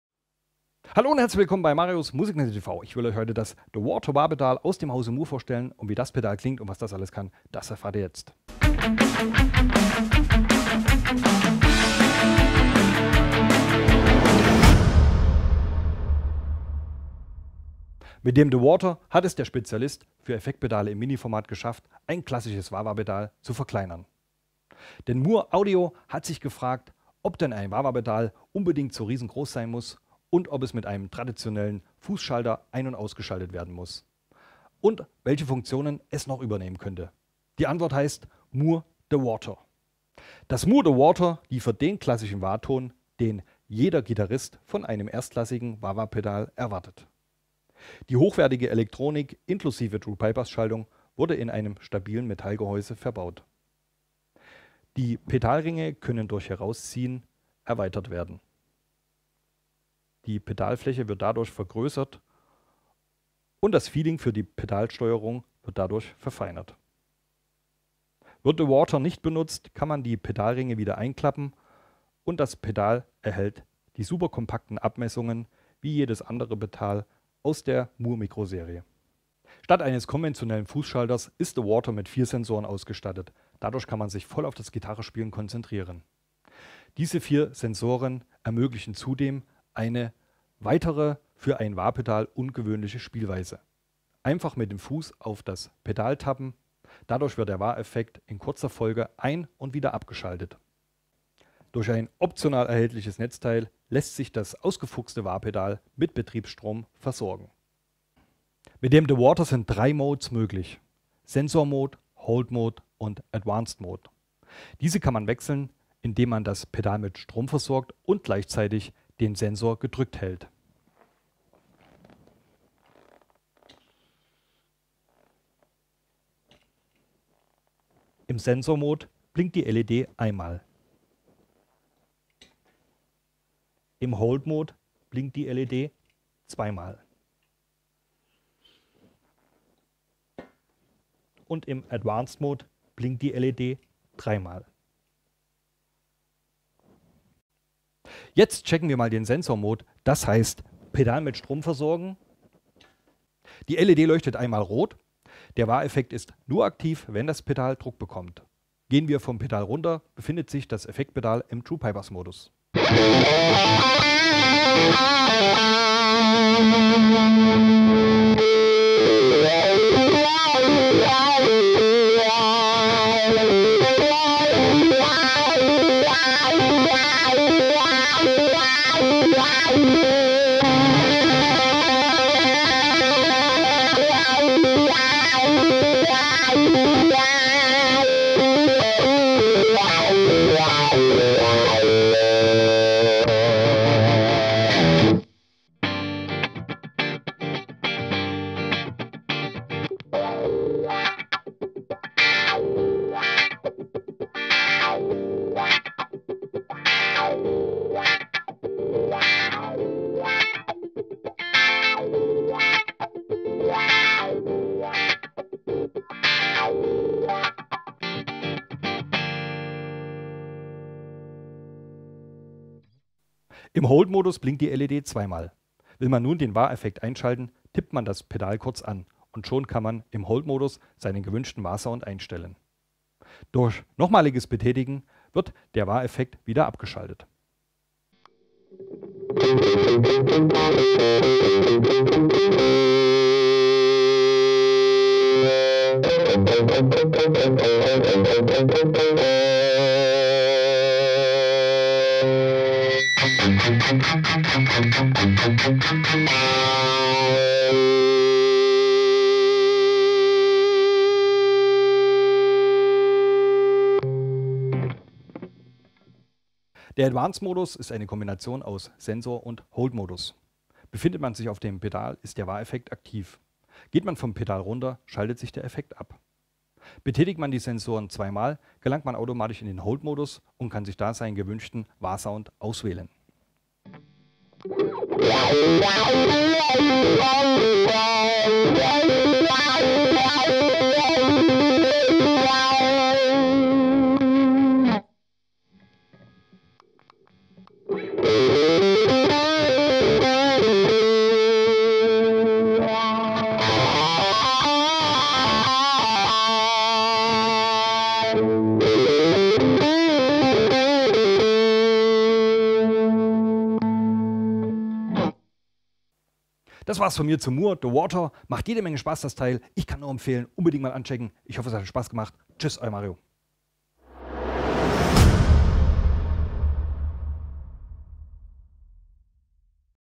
Mooer The Wahter - Review
Mooer The Wahter getestet von mit einer PRS Custom 24 über einem Blackstar Series One 50 mit Marshall Cabinet MR 1936 und abgenommen mit einem Shure SM57.